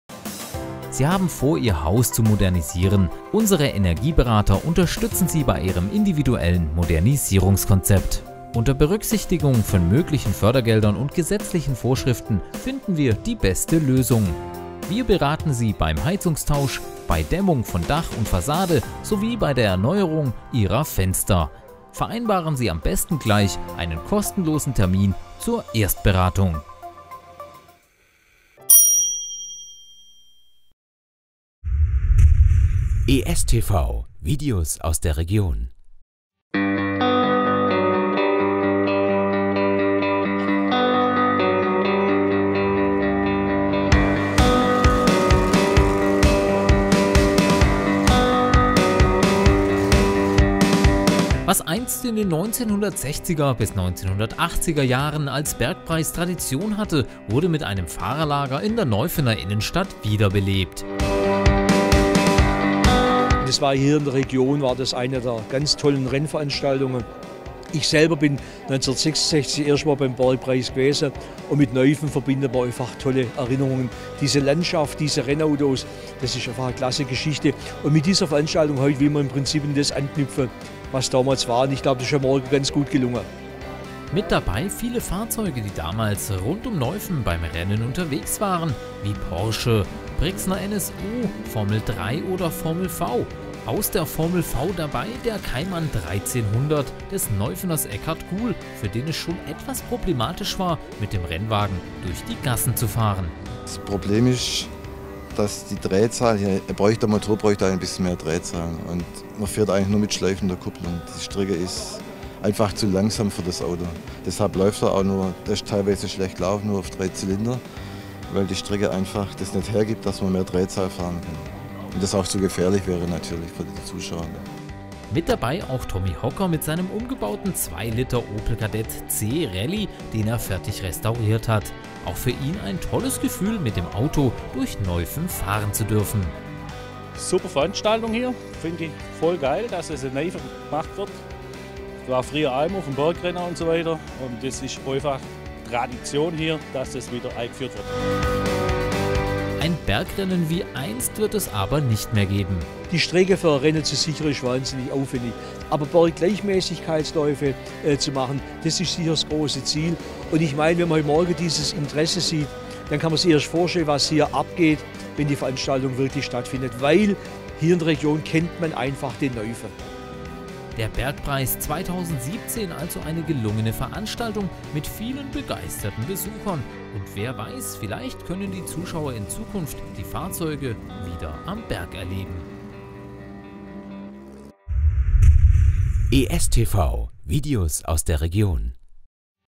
Bergpreis Neuffen
Jede Menge Nostalgie gab es am Sonntag in Neuffen beim Bergpreis. 40 Rennwagen, die in den 1960er bis 1980er Jahren beim Bergrennen unterwegs waren konnten in den Neuffener Gassen und bei einem Korso bestaunt werden.